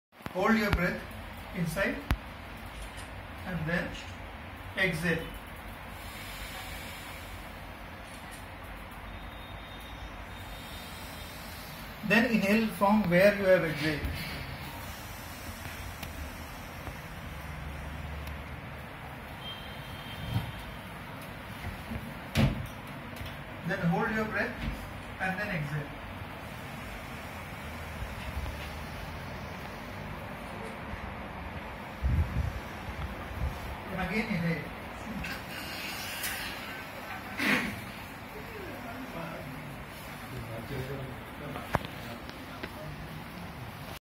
International yoga day 2017
Yoga is the way to connect with your being and soul. Arranged yoga session on this international yoga day at BIG IDEAS by Yoga Enthusiast Mr. Amit Sharma.